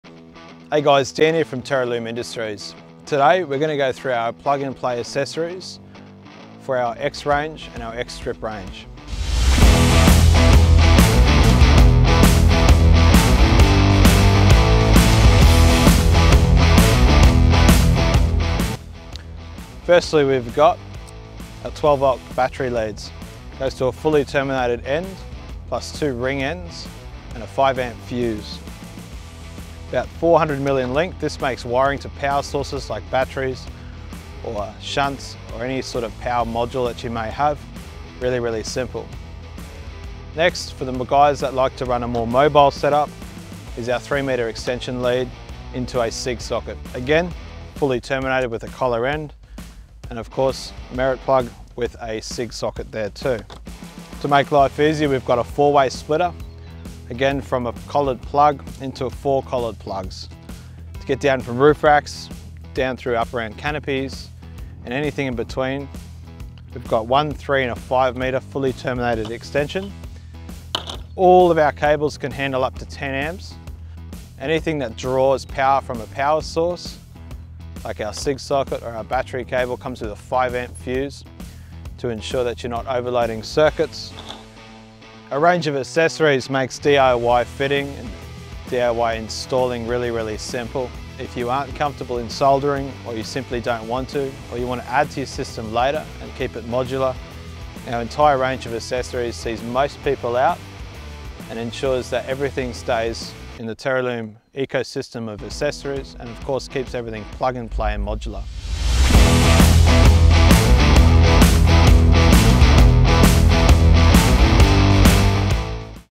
X-Range Cable & Extension Guide | Teralume Industries
Extension Cables – 2 Pin

1m Extension Cable - Fully Terminated
3m Extension Cable - Fully Terminated
5m Extension Cable - Fully Terminated

We’ve made life easier with our fully terminated extension cable range. If you’re not into soldering and just want the ease of plugging in and going, these extensions are for you! All of our accessories are interchangeable, meaning all our extension cables will work with our X-Strips and our X1 work light range.

Suits:
X1 Range of Work Lights
All X-Strip Lights
X-Range of Accessories (Splitter cables, dimmers and power accessories)

Splitter Cables

1 to 2-Way Power Splitter Cable
1 to 4-Way Power Splitter Cable

Make life easier with our power distribution splitter cables. 400mm long and fully terminated with our weather-sealed connectors, it will make distribution power to multiple lights a breeze! If you’re wanting to reduce cable runs, ensure to add a splitter cable to the list.

Plug & Play with the X-Range of Lighting and Accessories. Including X1, X-Strips, Battery Cable & Dimmer.

What's Included:
• 1x 400mm 2 Pin Splitter Cable - 1 to 2 way or 1 to 4 way

Power Cable – Battery Terminal:

The battery is where it all starts! This is our quick-connect power cable for our X-Range of products. Fully terminated and circuit protected with a 5amp inline fuse, all you have left to do is connect it to your battery.

What's Included:
1x 400mm Battery Cable Connector with inline 5amp fuse (included)

Power Cable – Cig Plug Socket 3 Meter

Plug in and go with our long 3-meter cigarette lighter power cable. Our power cable is fully terminated with a cigarette socket/merit plug and our weather-sealed connectors which suit the entire X-Range of work lights and strip lights. Our cable will bring a whole new level of portability to your work lights!

Plug & Play with the X1 Work Light Range & X-Strip Range and Accessories.

What's Included:
• 1x 3m 12V Cigarette Lighter Power Cable - 2 Pin Plug Suit X-Range